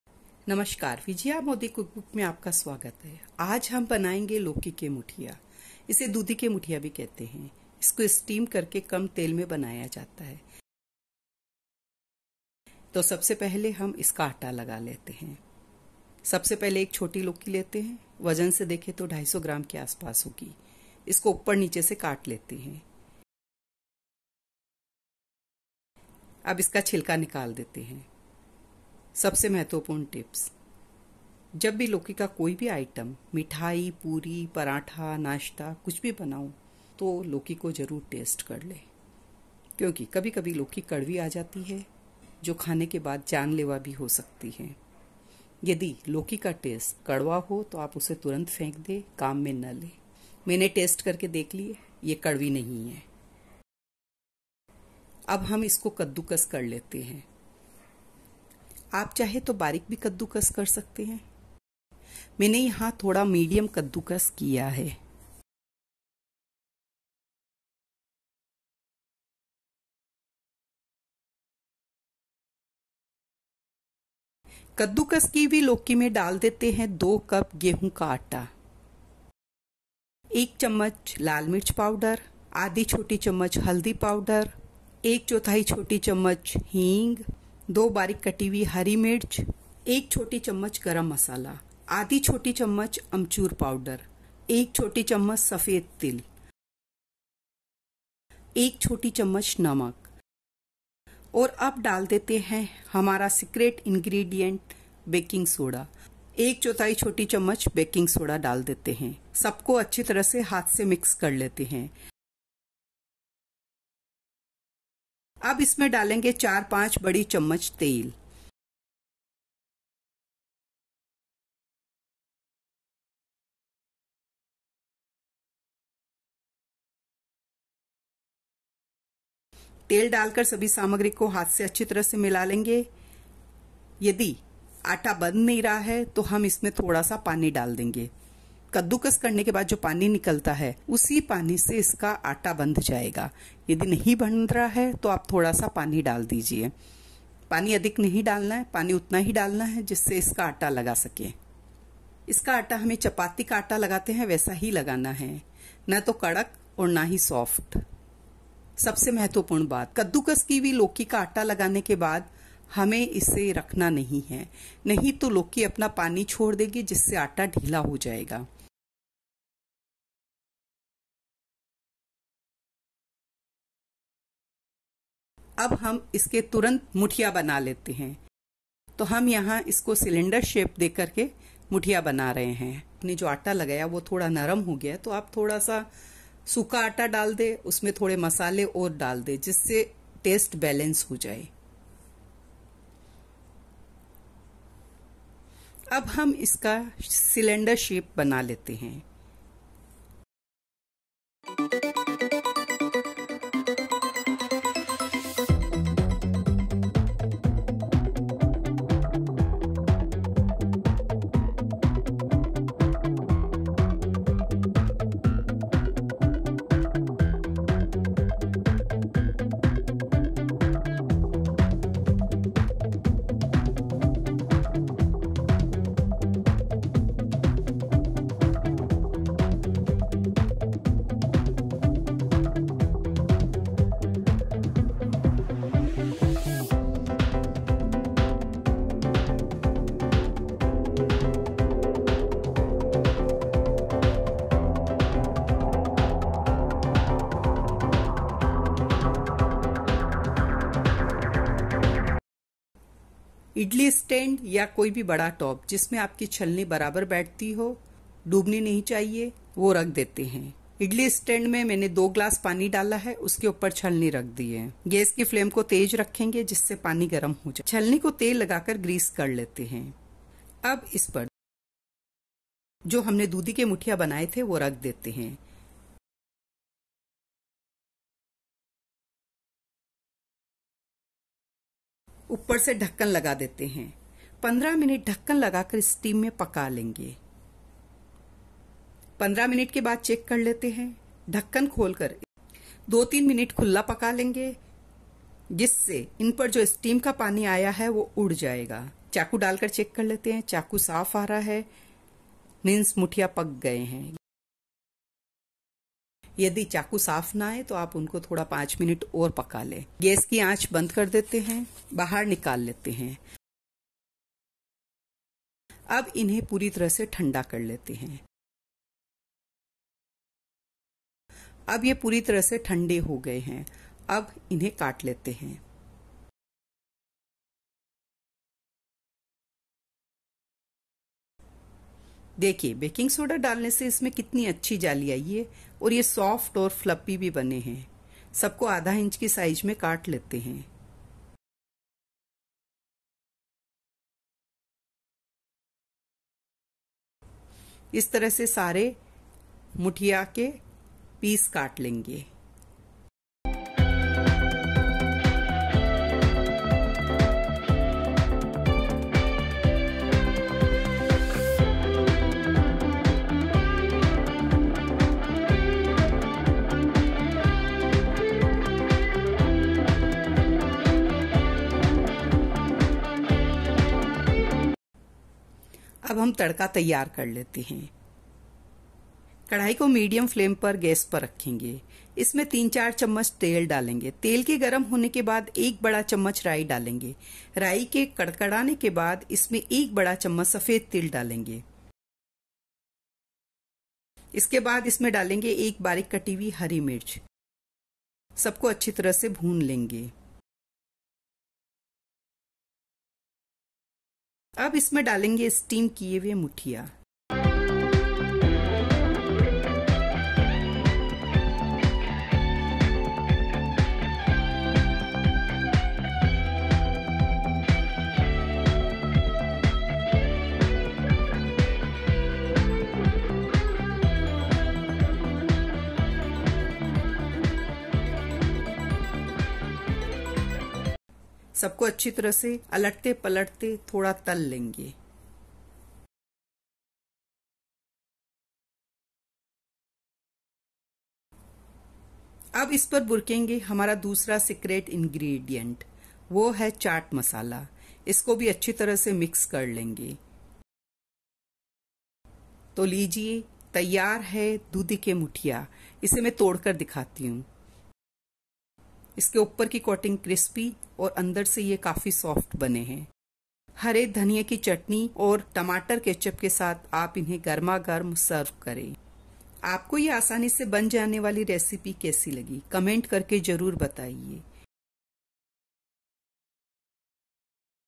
गेहूँ के आटे से बना हेल्दी और टेस्टी नाश्ता जो भी खाये,पहचान ना पाये। Breakfast Recipe
लौकी और आटे से बना हेल्दी और टेस्टी नाश्ता जो भी खाये, पहचान ना पाये।  Lauki ke Muthiya Recipe

लौकी से आसानी से बनाया जानेवाला नाश्ता जो बडो के साथ साथ बच्चो को बहुत पसंद आयेगा। इस नाश्ते की खास बात यह की जो बच्चे लौकी खाने से दूर भागते है उनको यह पता भी नहीं चलेगा की यह नाश्ता किस चीज से बनाया गया है।  यह नाश्ता खाने मे हेल्दी और टेस्टी भी है। इसको आटा, लौकी, मसाले और तिल डाल कर बनाया गया है । जो लोग प्याज़  लहसुन नही खाते उनके लिए  यह एक अच्छा नाश्ता है। कम तेल से बना यह नाश्ता आपको जरूर पसंद आयेगा।
Happy Cooking 😊
- Vijaya Modi's Cookbook